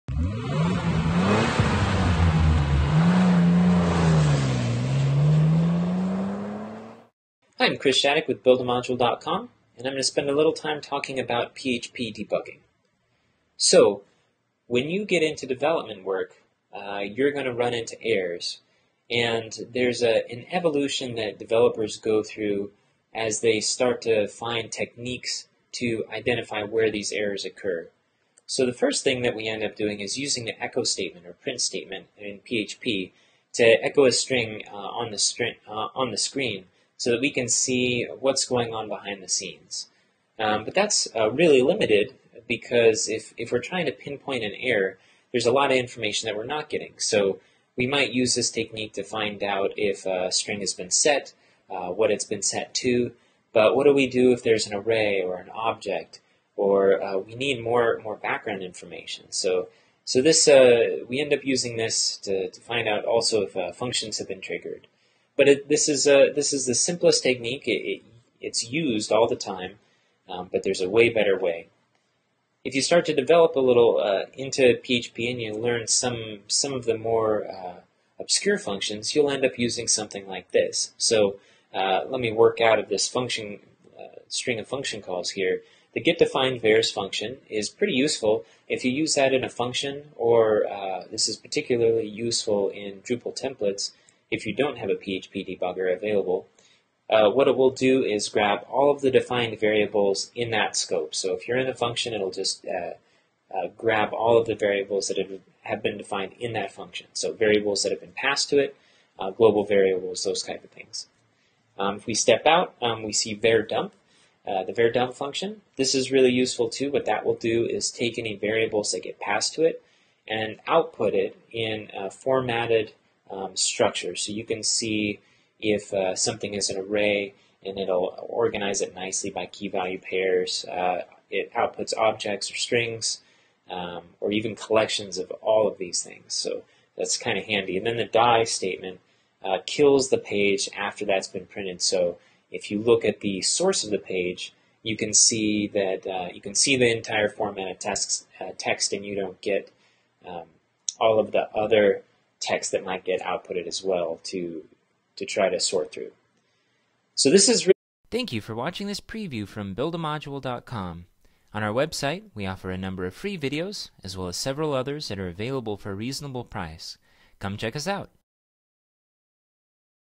Using a PHP Debugger to Debug Your Drupal Module
About this video:

As you develop, you will inevitably run into errors in your code. It can be a huge pain to track down obscure problems, or try to follow the values of variables through an entire process, and you. Using a PHP debugger is the ultimate solution. In this video, I will demonstrate a PHP debugger in action and explain the different components you'll need to get a debugger working in your development environment. Trust me, if you don't use a PHP debugger, you can save yourself hours by watching this video.